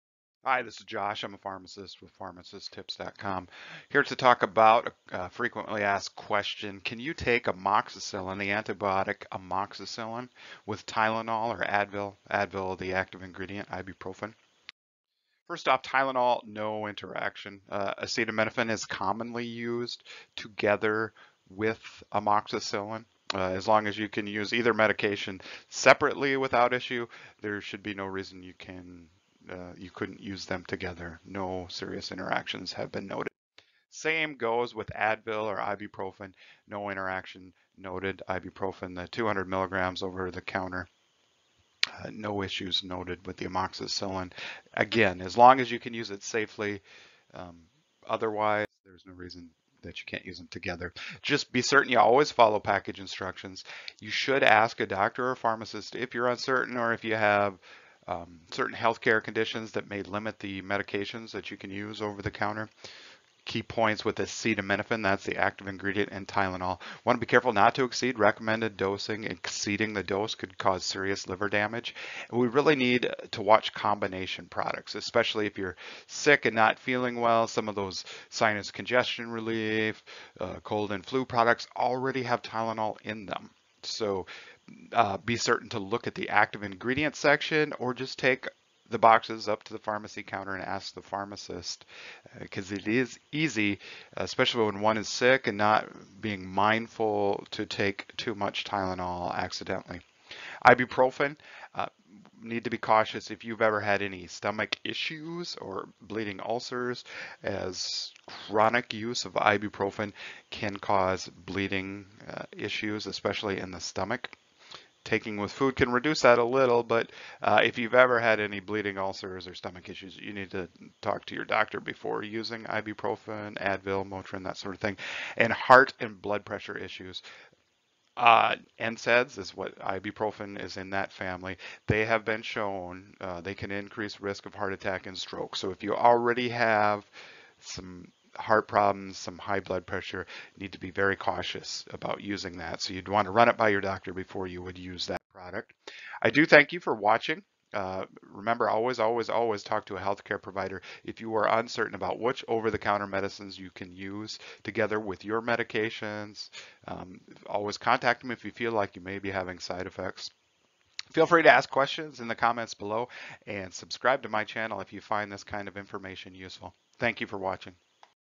Can you take Amoxicillin with Tylenol or Advil ibuprofen
Taking Tylenol (acetaminophen) or Advil (ibuprofen) with amoxicillin is discussed.